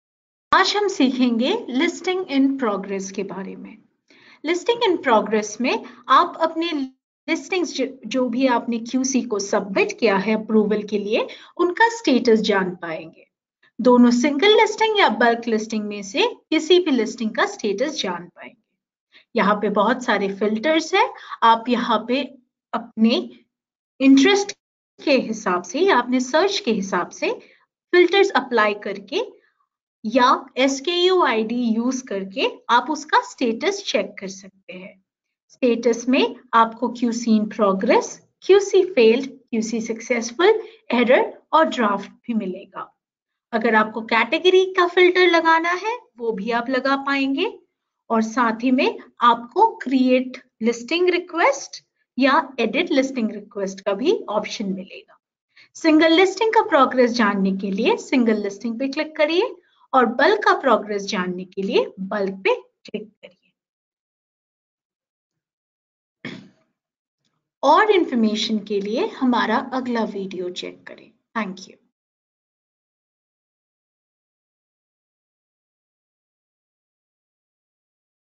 What is Listings in progress on Flipkart I Step by step guide from Flipkart I Hindi
Understand the listing on Flipkart through this simple step by step guide video.

Have a look at this video and find out more!

Become a Seller now